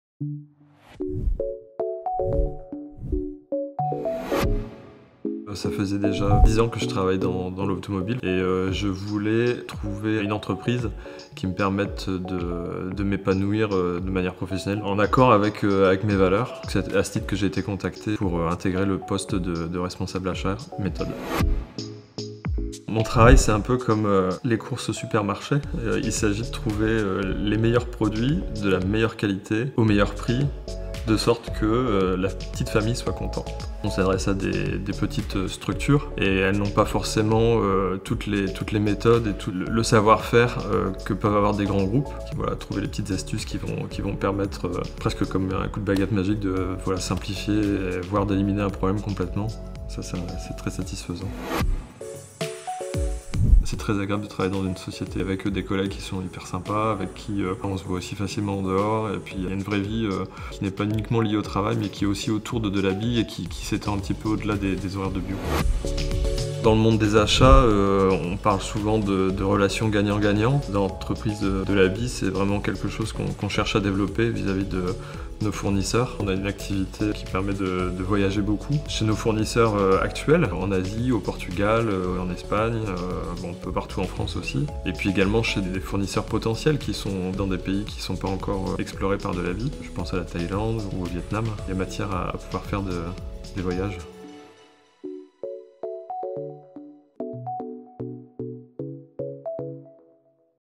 DELABIE Inside - Épisode 8 : Pierre, Responsable achats-méthodes
DELABIE Inside - Épisode 8 : Découvrez le métier de Responsable achats-méthodes chez DELABIE en 90 secondes ! Pierre nous explique son métier au sein de l’entreprise : « il s’agit de trouver les meilleurs produits, de la meilleure qualité, au meilleur prix ».